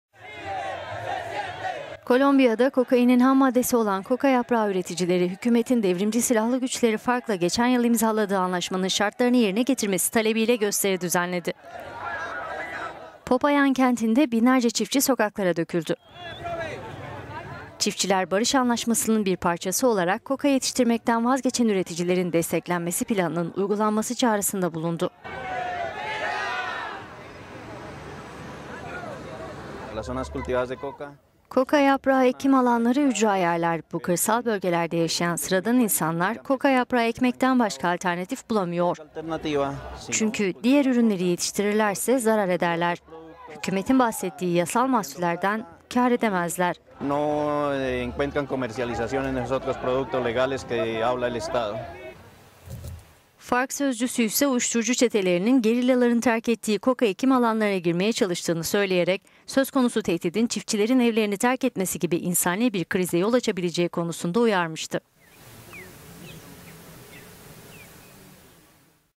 Kolombiyalı koka yaprağı üreticileri sokaklara döküldü
Kolombiya'da kokainin hammadesi olan koka yaprağı üreticileri hükümetin Devrimci Silahlı Güçleri (FARC) ile geçen yıl imzaladığı anlaşmanın şartlarını yerine getirmesi talebiyle gösteri düzenledi. Popayan kentinde binlerce çiftçi sokaklara döküldü.

Çiftçiler barış anlaşmasının bir parçası olarak koka yetiştirmekten vazgeçen üreticilerin desteklenmesi planının uygulanması çağrısında bulundu: "Koka yaprağı ekim alanları ücra yerler. Bu kırsal bölgelerde yaşayan sıradan insanlar koka yaprağı ekmek…